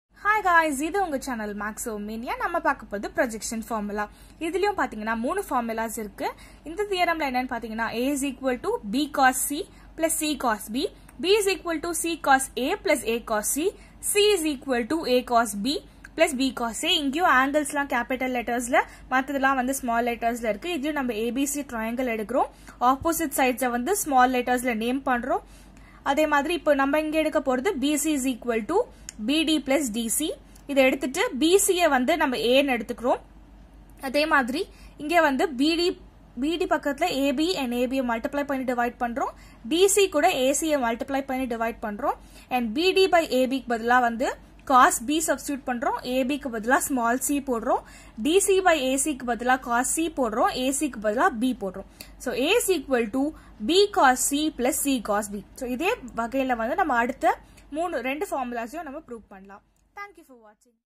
PROJECTION FORMULA WITH PROOF IN TAMIL
Hi guys... in this video you will understand about projection formula in tamil 🔥